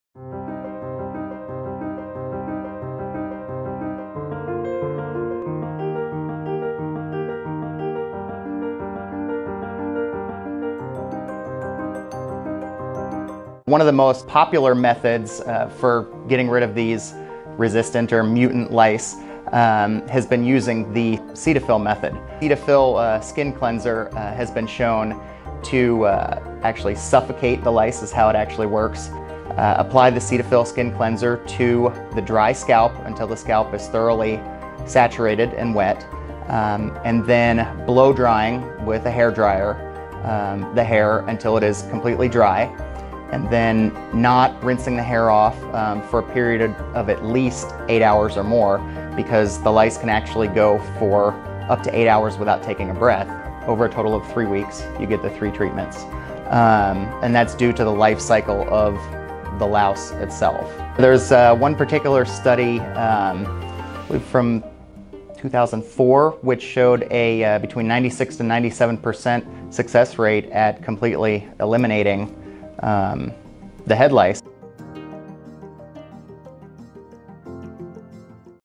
Household product safely cures drug resistant head lice
Massachusetts is just one of the 25 states that have had reports of  mutant lice.   These lice have become resistant to the commonly used over-the-counter products and even some prescription drugs.

Below Dr. Michael Klatte, infectious disease specialist at Baystate Children’s Hospital, talks about how to protect your child from lice and highlights a way to safely and effectively eliminate any type of lice without harsh chemicals.